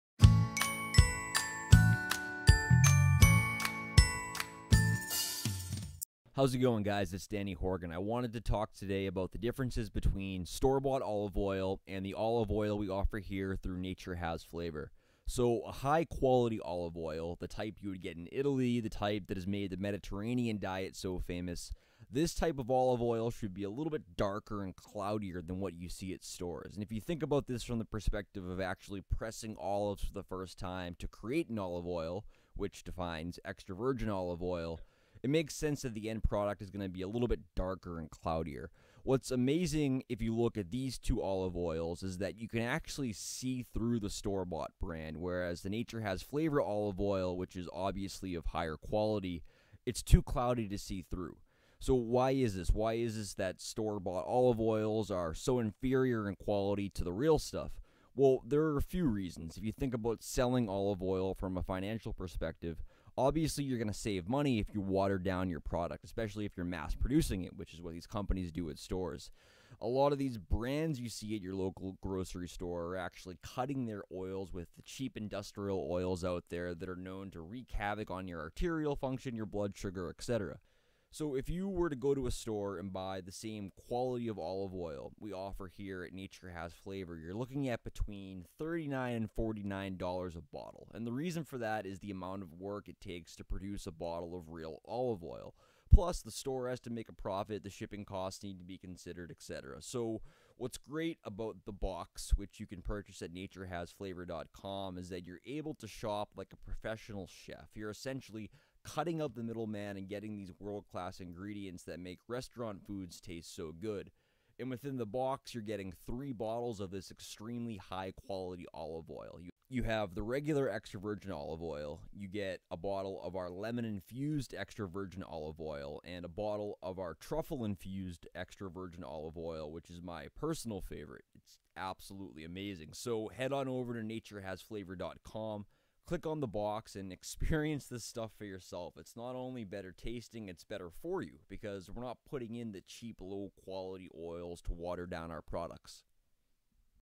Real Extra Virgin Olive Oil vs. Store-Bought Brands
The difference between real extra virgin olive oil and store-bought brands cut with food coloring is immense. We've sourced our extra virgin olive oil in small batches from Italy to ensure you are getting the real thing -- no fillers, no surprises. Get your own Nature Has Flavor extra virgin olive oil Your friends and family will thank you!